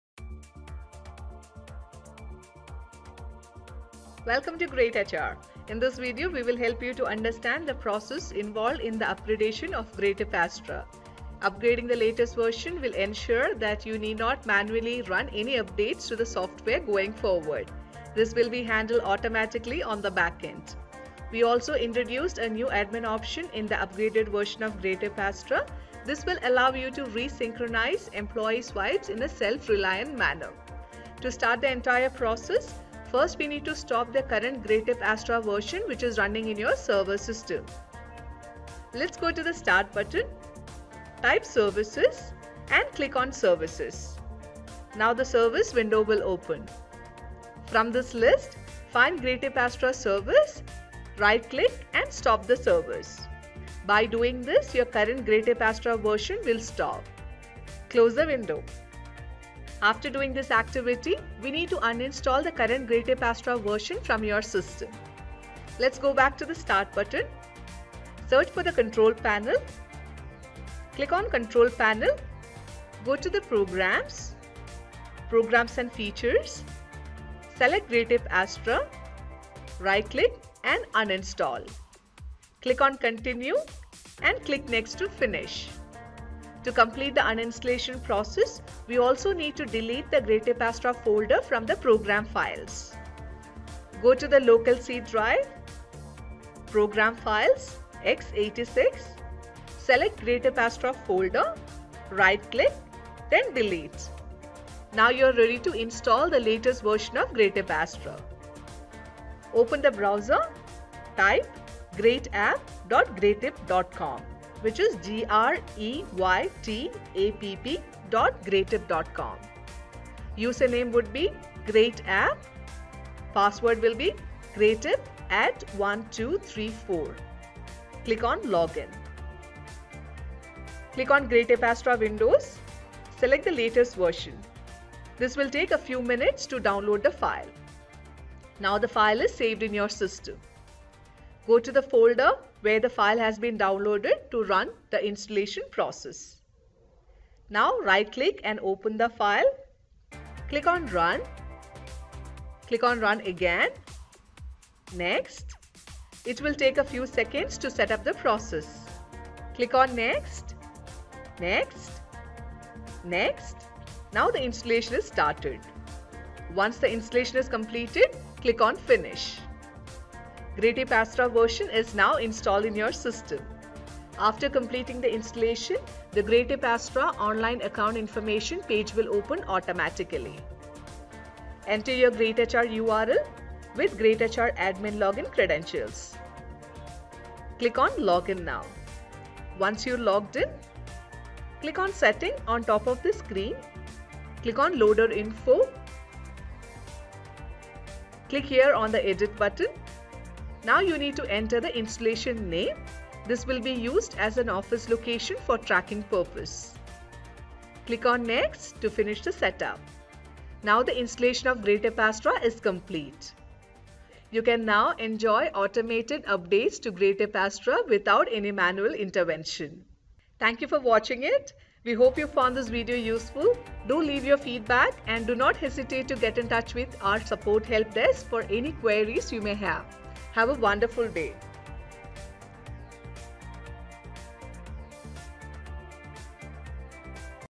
Upgrade to New Greytip Astra Version
Greytip Astra offers a seamless process for upgrading to the latest version, ensuring that you need not manually run any updates going forward. The new version includes an automatic update feature, which simplifies software maintenance on the backend. Additionally, an admin option to re-synchronize employees' swipes has been introduced, enabling self-reliant management of attendance data. This video provides a comprehensive guide to the upgrading process and the new features of Greytip Astra.